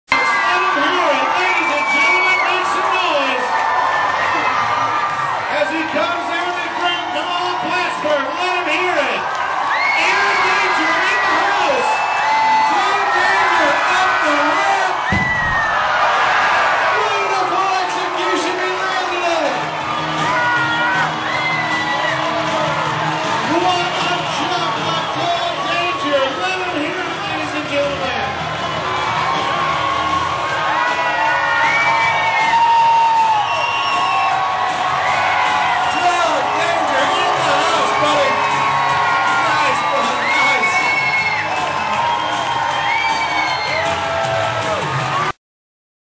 Doug Danger Motorcycle Jump 2014 at Clinton County Fairgounds in Plattsburgh NY
7/26/14 KSR Motorsports